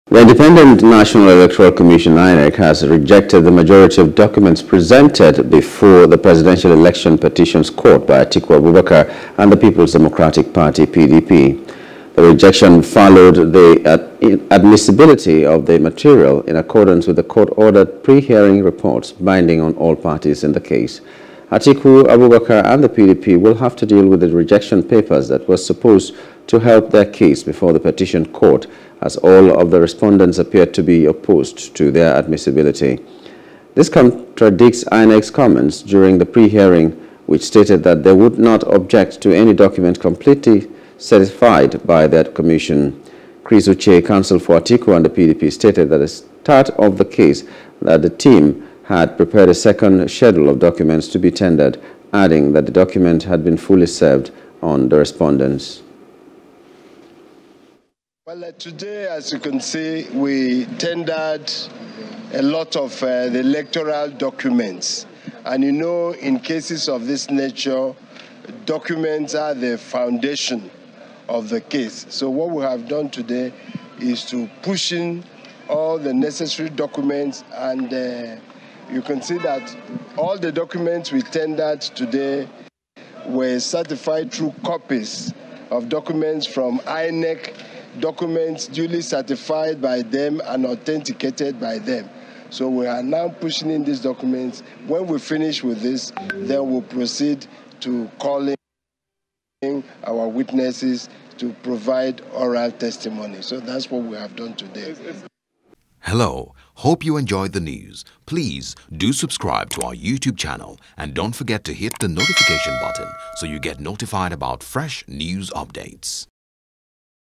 Election Tribunal: INEC Rejects Certified True Copies Of Documents Tendered By Atiku, PDP
The Independent National Electoral Commission, INEC, has rejected the majority of documents presented before the Presidential Election Petition Court by Atiku Abubakar and the People’s Democratic Party, PDP.